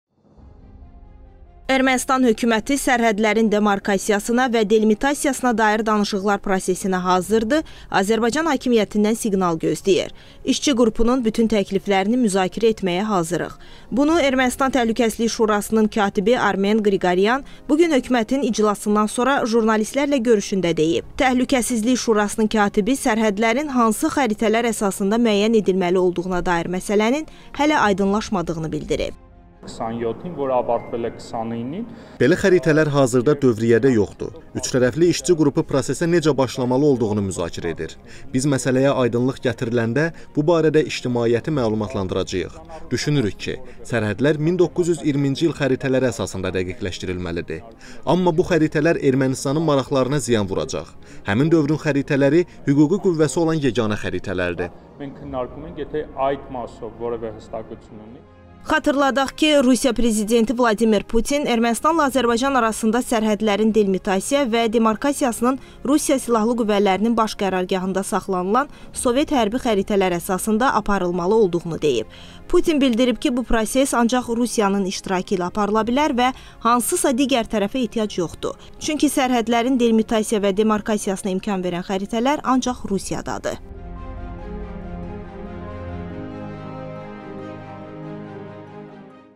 Ermənistandan sərhədlə bağlı mühüm açıqlama - Baku TV
Ermənistan hökuməti sərhədlərin demarkasiyasına və delimitasiyasına dair danışıqlar prosesinə hazırdır, Azərbaycan hakimiyyətindən siqnal gözləyir. İşçi qrupunun bütün təkliflərini müzakirə etməyə hazırıq.
Bunu Ermənistan Təhlükəsizlik Şurasının katibi Armen Qriqoryan bu gün hökumətin iclasından sonra jurnalistlərlə görüşündə deyib.
Təhlükəsizlik Şurasının katibi sərhədlərin hansı xəritələr əsasında müəyyən edilməli olduğuna dair məsələnin hələ aydınlaşmadığını bildirib.
Xatırladaq ki, Rusiya Prezidenti Vladimir Putin Ermənistanla Azərbaycan arasında sərhədlərin delimitasiya və demarkasiyasının Rusiya Silahlı Qüvvələrinin Baş Qərargahında saxlanılan sovet hərbi xəritələri əsasında aparılmalı olduğunu deyib. Putin bildirib ki, bu proses ancaq Rusiyanın iştirakı ilə aparıla bilər və hansısa digər tərəfə ehtiyac yoxdur. Çünki sərhədlərin delimitasiyası və demarkasiyasına imkan verən xəritələr ancaq Rusiyadadır.